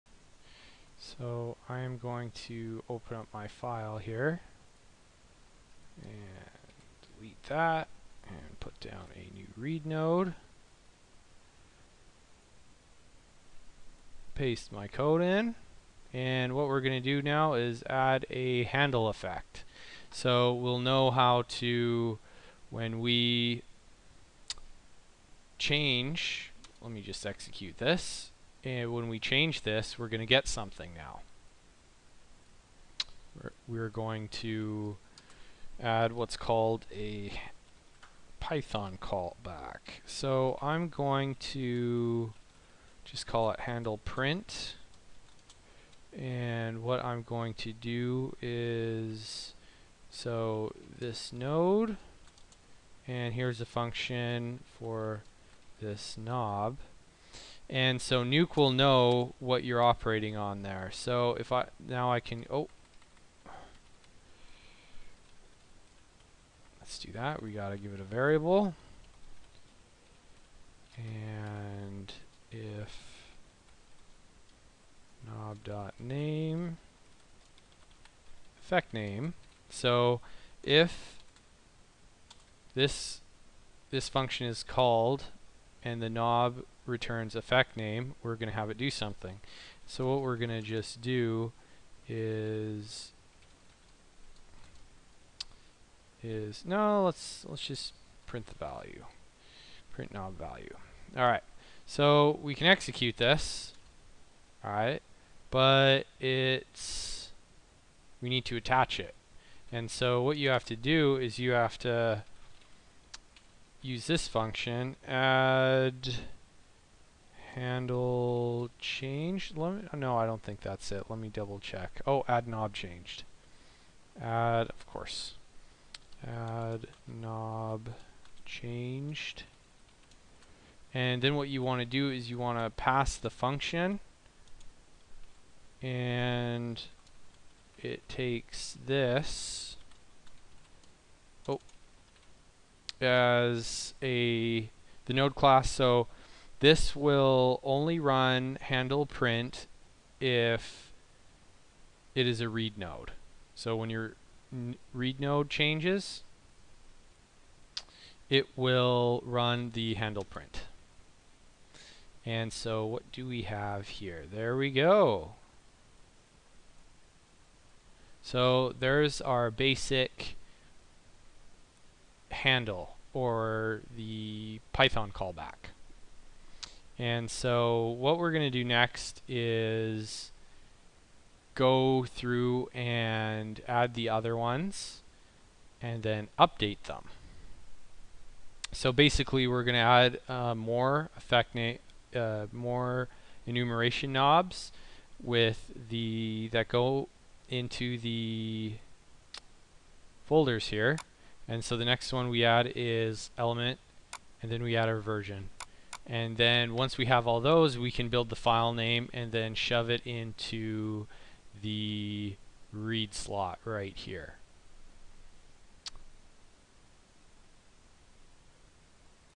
03 IntroToCallbacks
Adding custom python commands in nuke